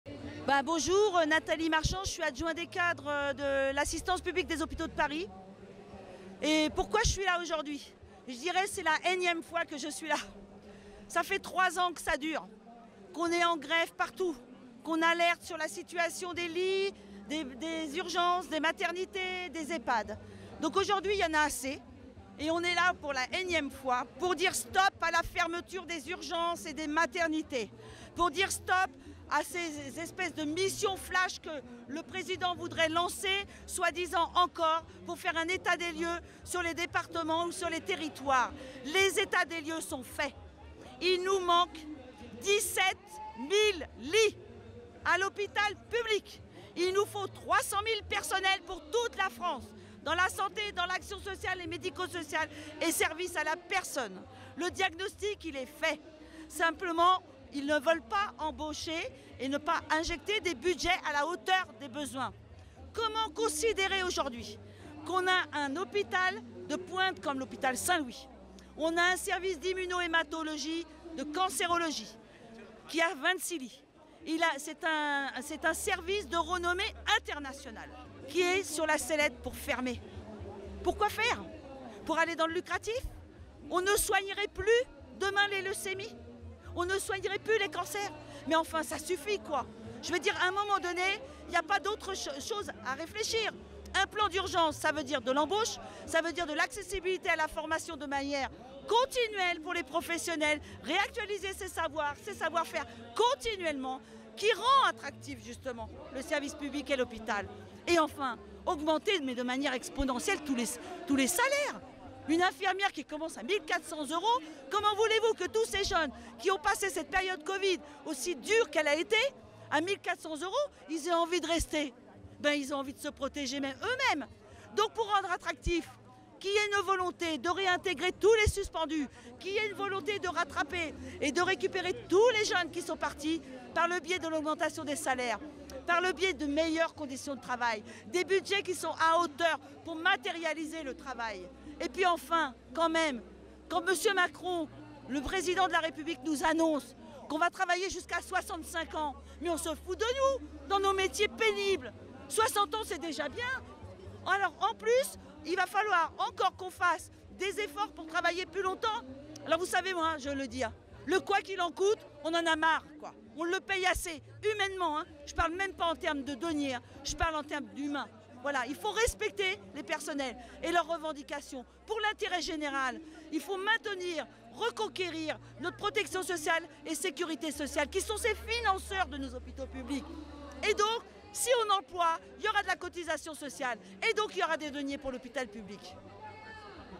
Il manque 17.000 lits, 300.000 personnes dans l’hôpital public. Dimanche on vote pour que ça change
Retour sur la mobilisation sur la santé mardi 7 juin